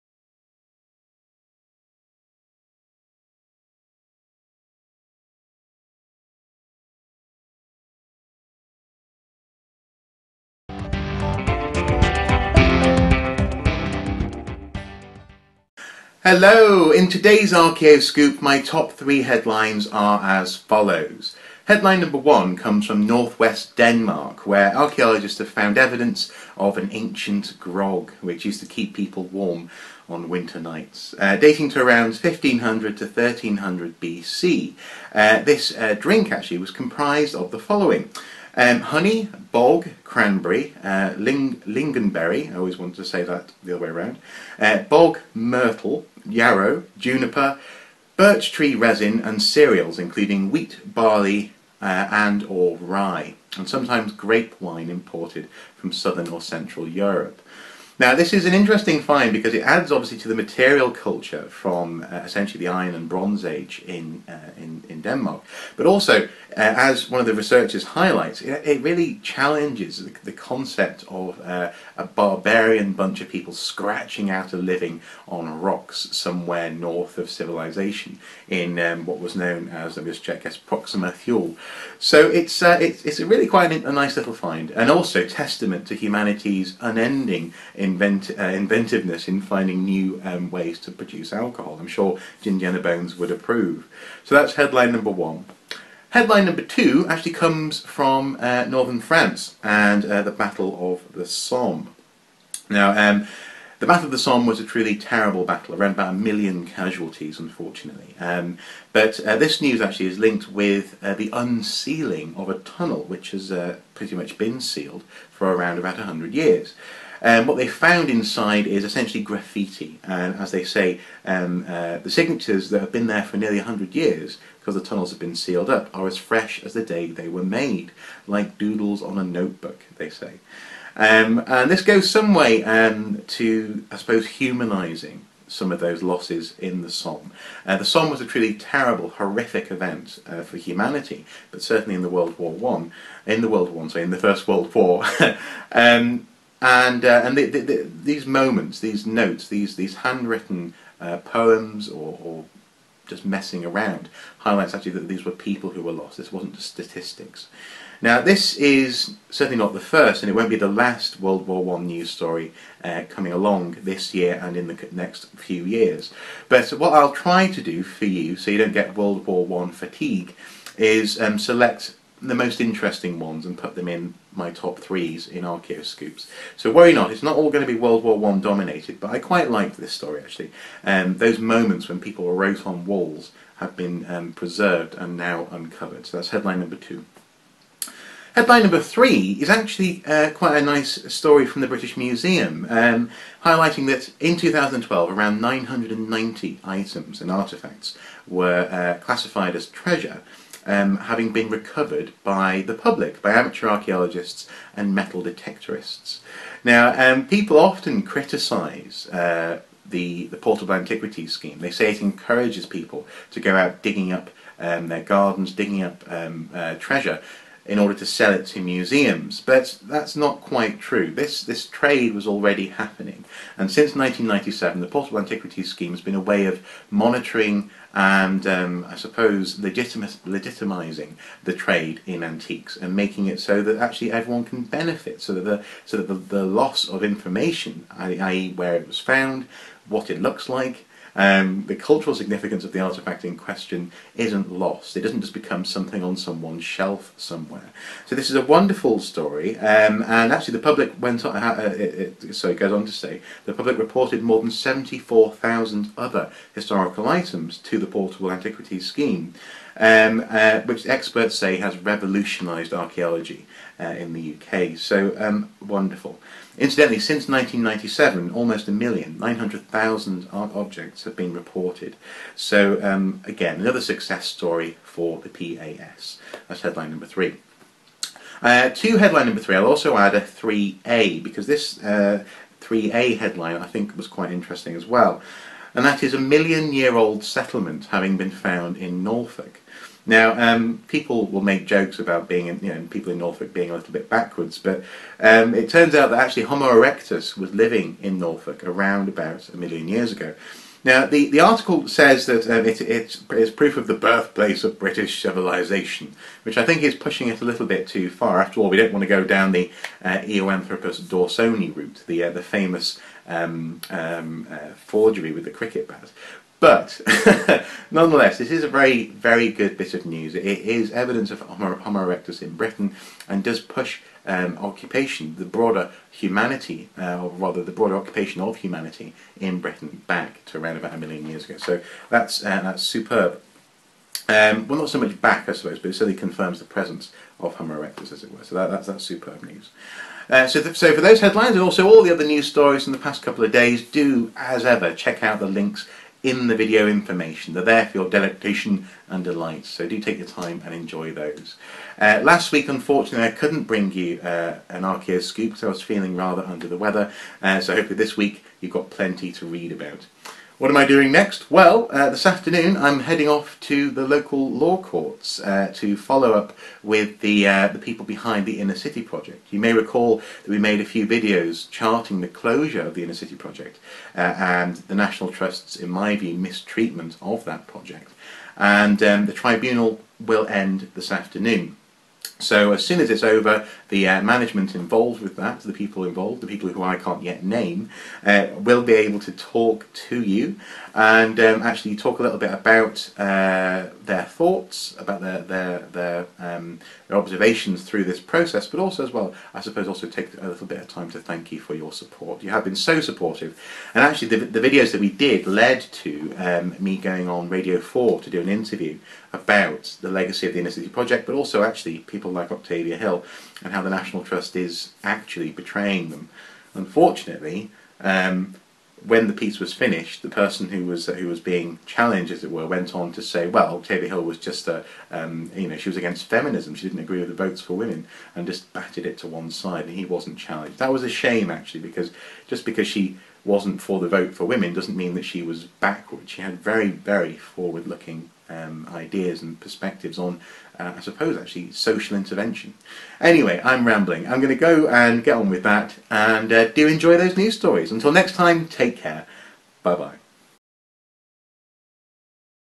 Archaeoscoop: Scandinavian Iron Age Grog
Welcome to Archaeoscoop, the place to find heritage and archaeology related news stories from around the world!

Today we hear about Grog in Iron Age Scandinavia, Doodles in the Somme and another success story for the PAS...

Britain's Oldest Chapel to be Excavated: Archaeologists to explore C6th Cornish Church built by Saint Piran: